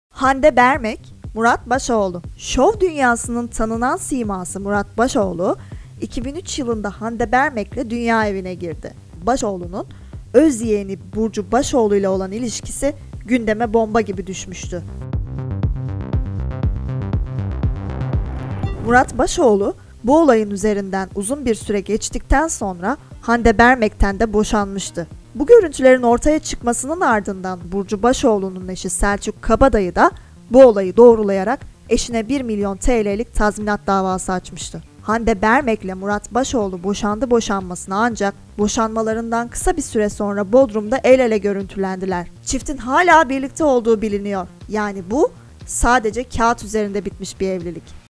MURAT BAŞOĞLU'NUN SKANDALLARLA DOLU BOŞANMA OLAYI
Öz yeğeni ile yakalanan Murat Başoğlu'nun gündeme bomba gibi düşen boşanma olayı!